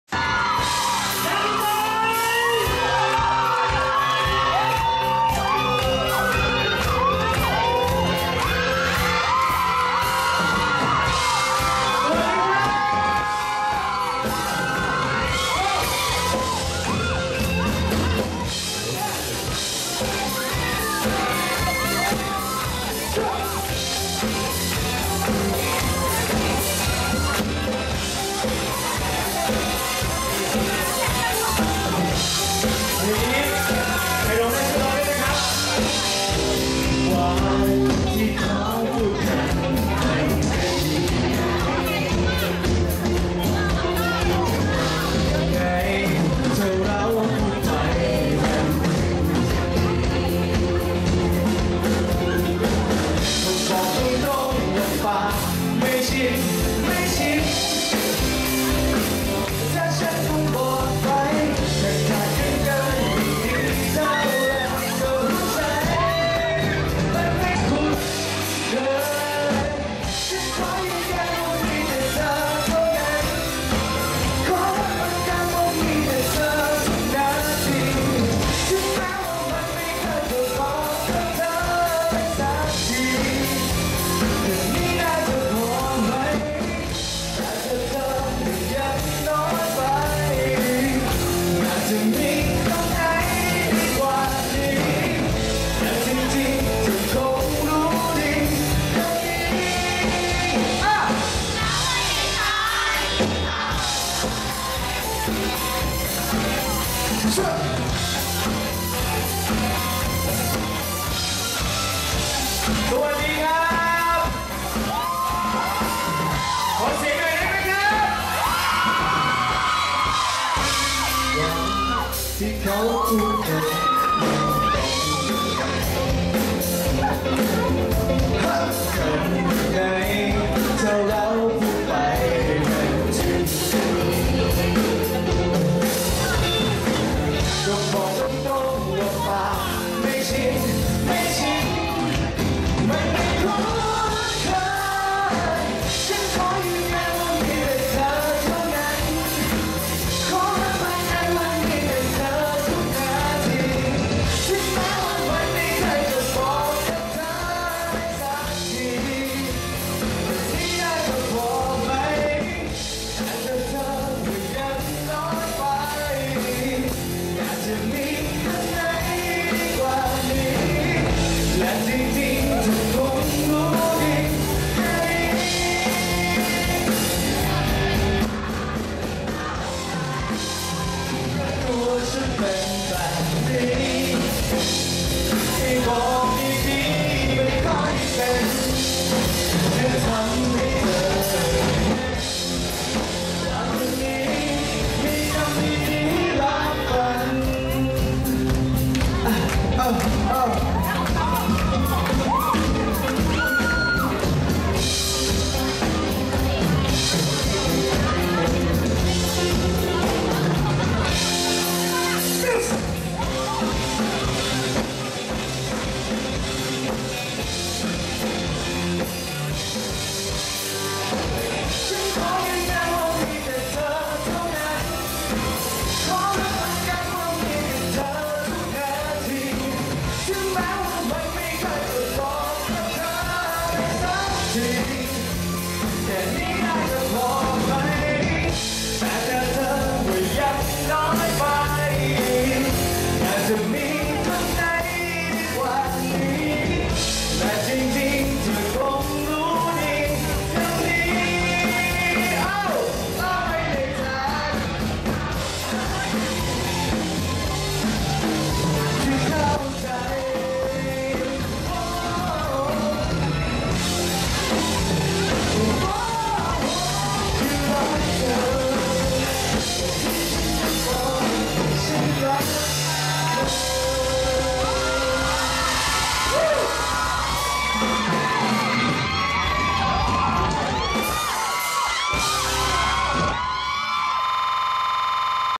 Aof2nd#01ละไว้ในฐานที่เข้าใจ
มินิคอนเดี่ยว ครั้งที่ 2
ออฟ ชาญณรงค์  หอมชิต
INCH รัชดา
วันอาทิตย์ที่ 27 กุมภาพันธ์ 2554